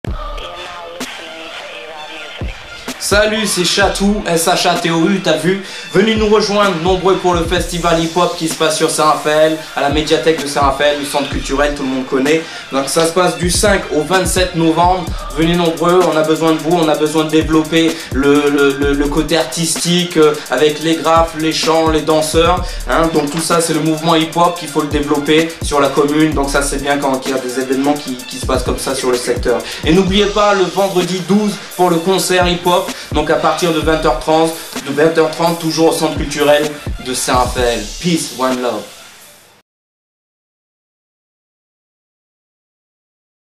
HHF video#01 Shatou01.mpg
Vidéo de présentation du HIP-HOP FESTIVAL par SHATOU